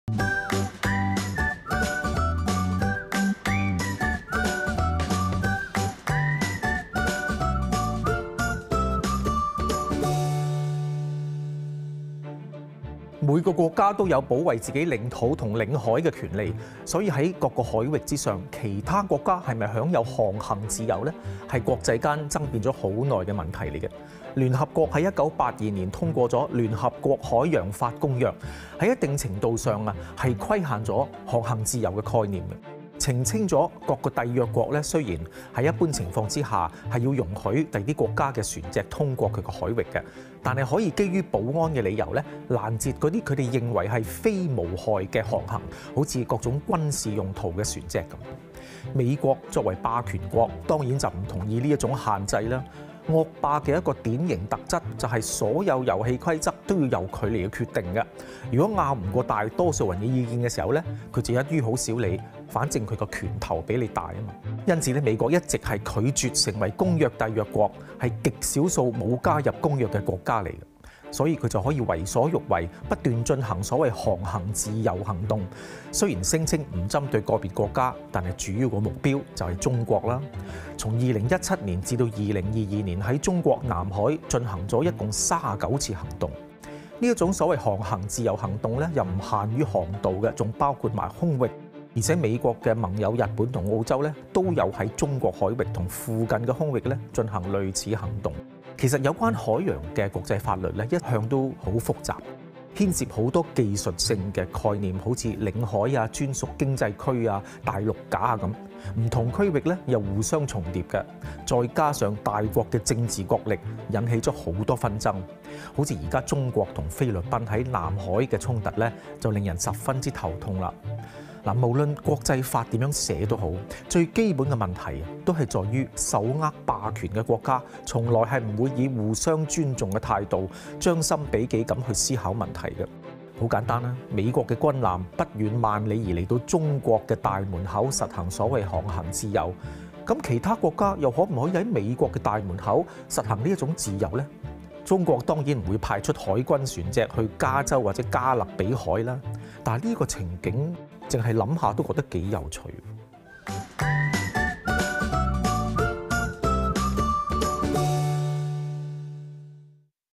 有理說得清｜惡霸的自由｜楊立門
每個國家都有保衞自己領土和領海的權利，所以在各個海域上其他國家是否享有航行自由，是國際間爭辯了很久的問題。聯合國在1982年通過《聯合國海洋法公約》，在一定程度上規限了航行自由的概念，澄清了各締約國雖然在一般情況下要容許別國的船隻通過其海域，但可以基於保安理由欄截那些他們認為是「非無害」的航行，例如是各種軍事用途的船隻。 美國作為霸權國，當然不同意這種限制。惡霸的一個典型特質，就是所有遊戲規則都要由他來決定，若拗不過大多數人的意見時，他便一於少理，反正他的拳頭比你的大。美國一直拒絕成為《公約》締約國，是極少數沒有加入《公約》的國家，所以他就可以為所欲為，不斷進行所謂「航行自由行動」，雖然聲稱不針對個別國家，但主要的目標就是中國。從2017 – 2022年在中國南海進行了一共39次行動。這種「航行自由行動」不限於航道，還包括空域，而且美國的盟友日本和澳洲也有在中國海域和附近的空域進行類似行動。 其實有關海洋的國際法律一向十分複雜，牽涉很多技術性的概念如領海、專屬經濟區、大陸架等，不同區域又互相重叠，再加上大國的政治角力，引起很多紛爭，如現在中國和菲律賓在南海的衝突，便令人十分頭痛。無論國際法怎麼寫，最基本的問題在於手握霸權的國家從來不會以互相尊重的態度，將心比己地思考問題。很簡單，美國的軍艦不遠萬里來到中國的大門口實行所謂「航行自由」，那麼其他國家可否在美國大門口實行這種自由呢？中國當然不會派出海軍船隻到加州海岸或加勒比海，但這個情景，光是想想也很有趣。